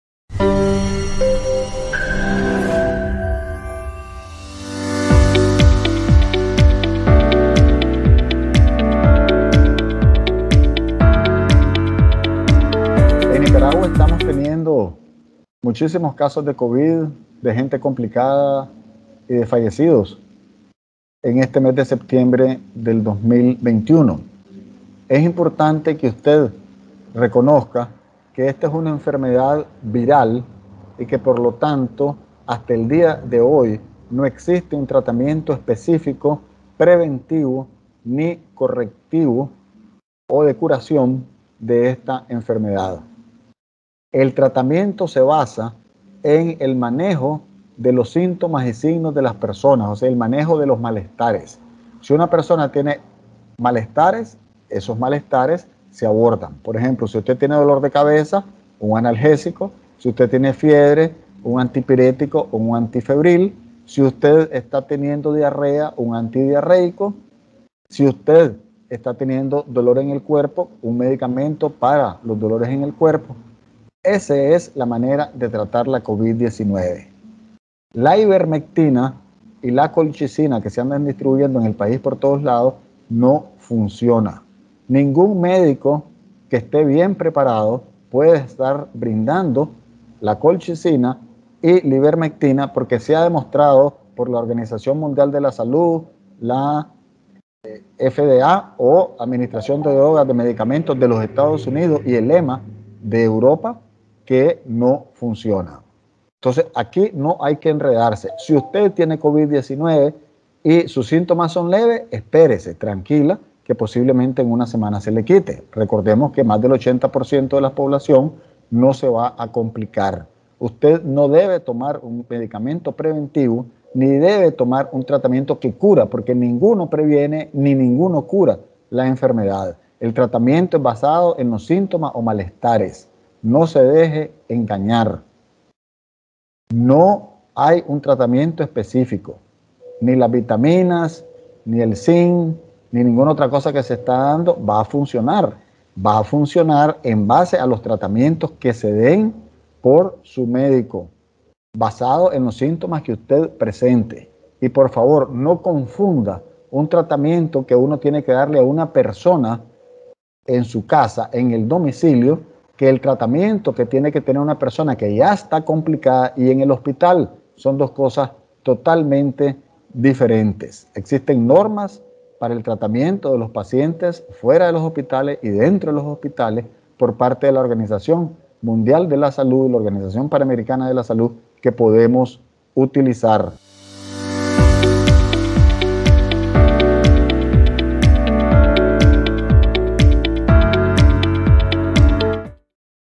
Si tiene síntomas gripales es mejor ir al médico | Radio Camoapa Estéreo
El epidemiólogo Leonel Argüello, quien se mantiene constantemente brindando recomendaciones para evitar el contagio de la covid-19, alertó que la ivermectina y la colchicina la distribuyen en Nicaragua, cuando ese medicamento no funciona ni para prevenir ni curar el coronavirus, informó en su portal digital Por tu salud Nicaragua.